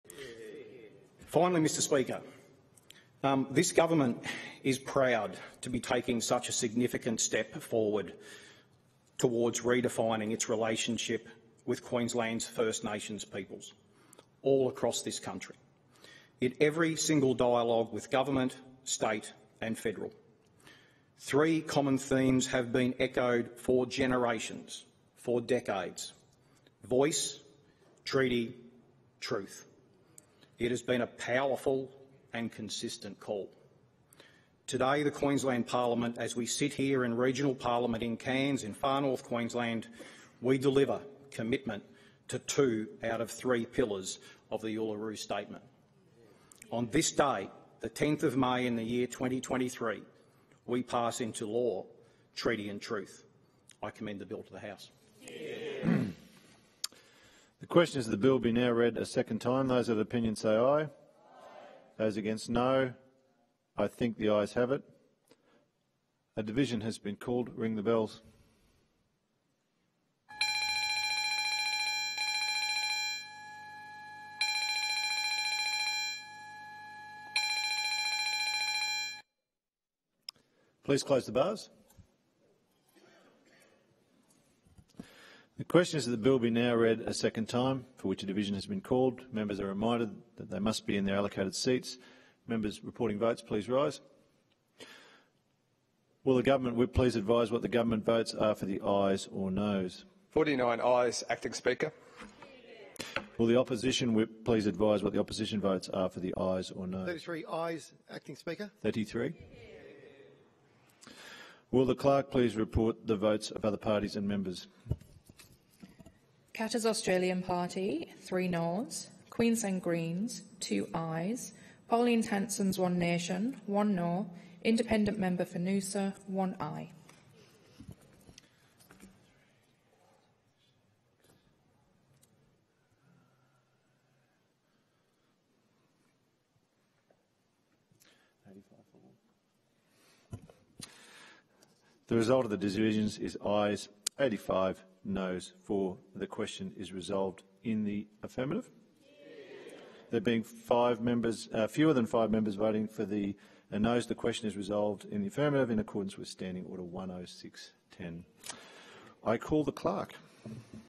How Qld MPs voted on Path to Treaty Bill
Extract of the Parliamentary Video showing the final breakdown on votes for Queensland's Path to Treaty Bill - 10 May 2023 in Cairns